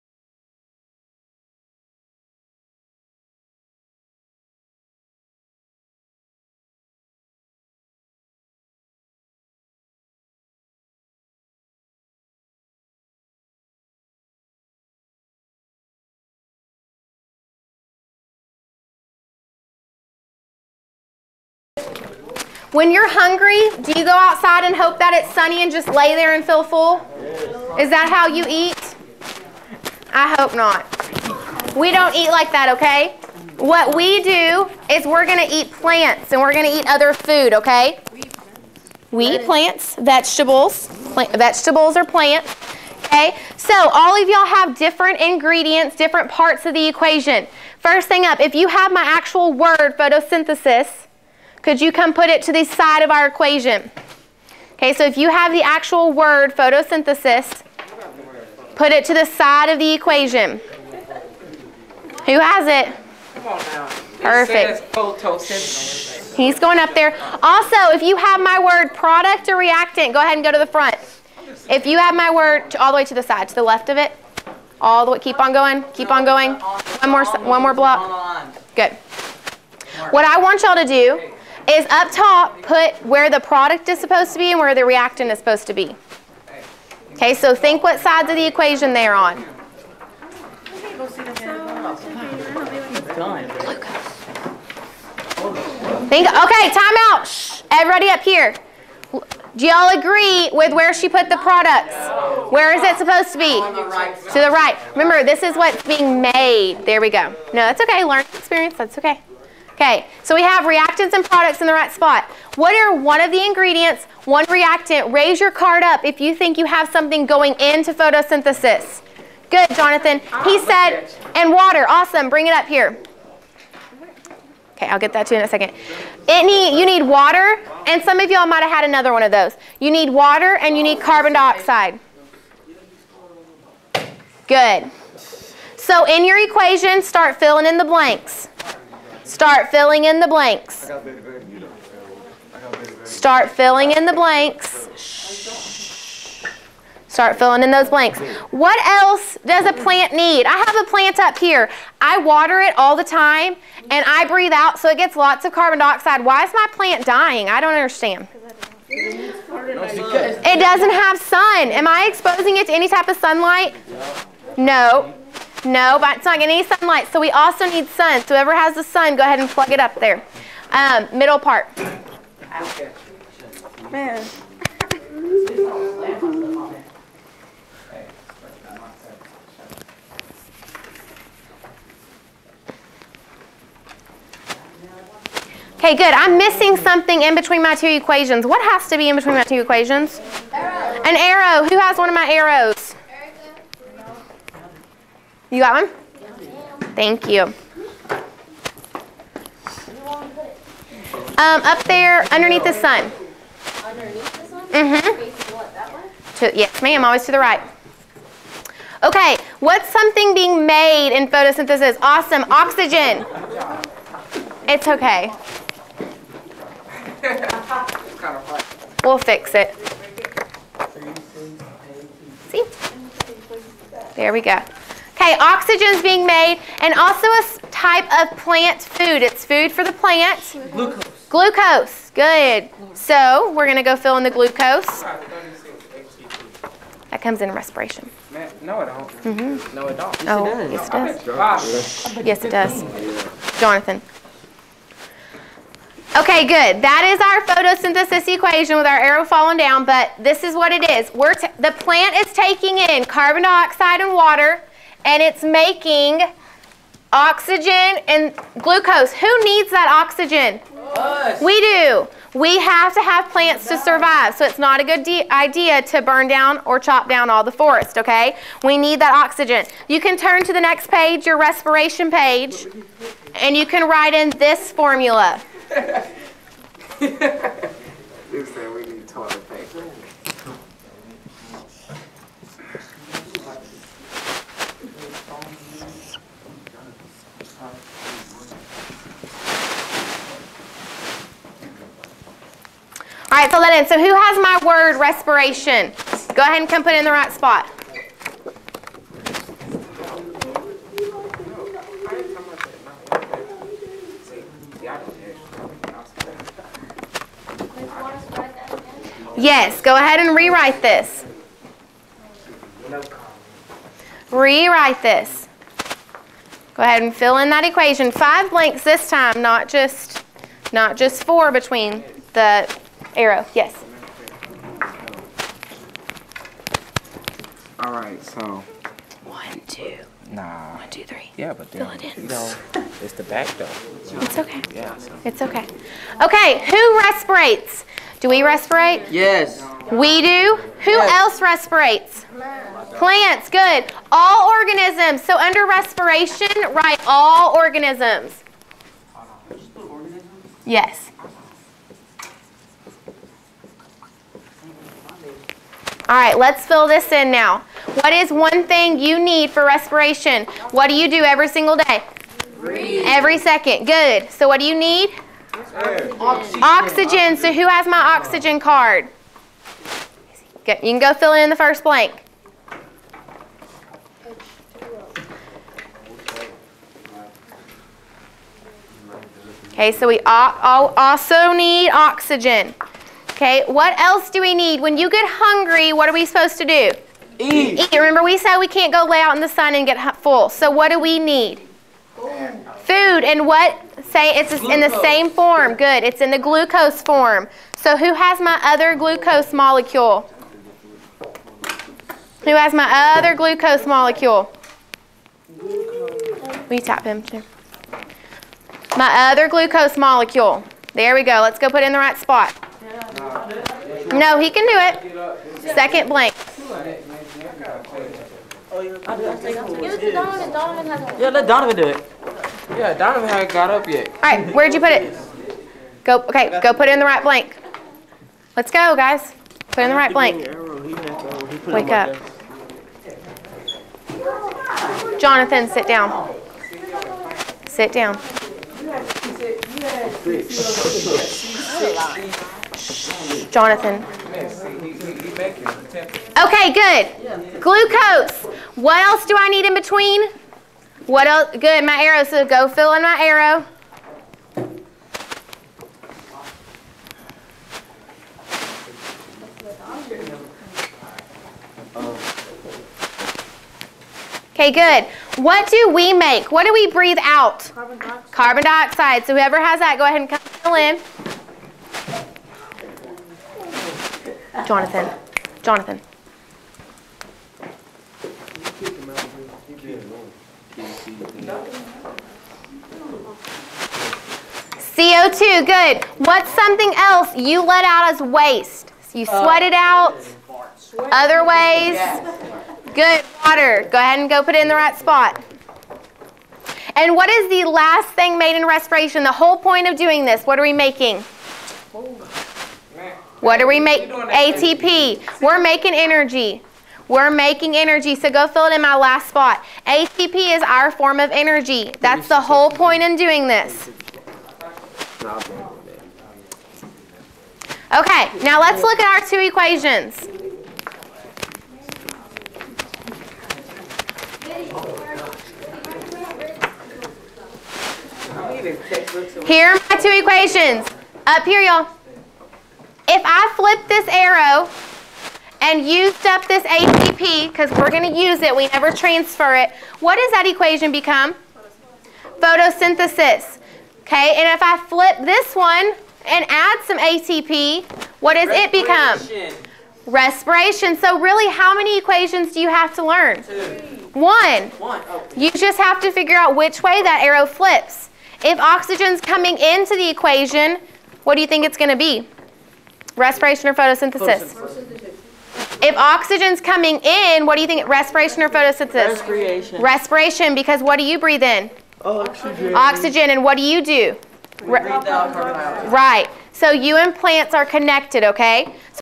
Biology Review: Learning about Photosynthesis
This video was Filmed at Mesquite High School on October 13, 2011. It is a Biology Campus Based Assessment 1st 6wks Review. There are 6 different sessions filmed. M6